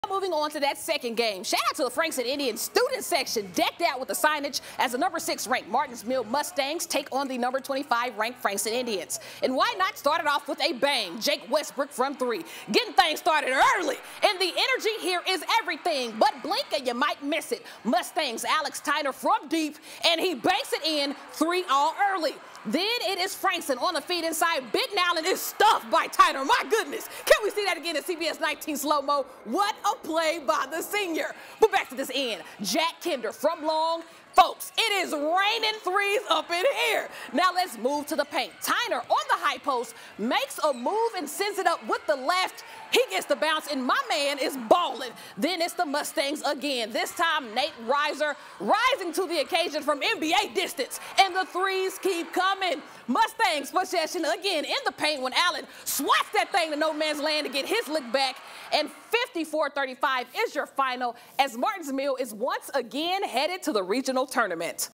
UIL Regional Quarterfinals: No. 6 Martin's Mill vs. No. 25 Frankston
The Martin's Mill Mustangs took on the Frankston Indians in an all East Texas, top 25 matchup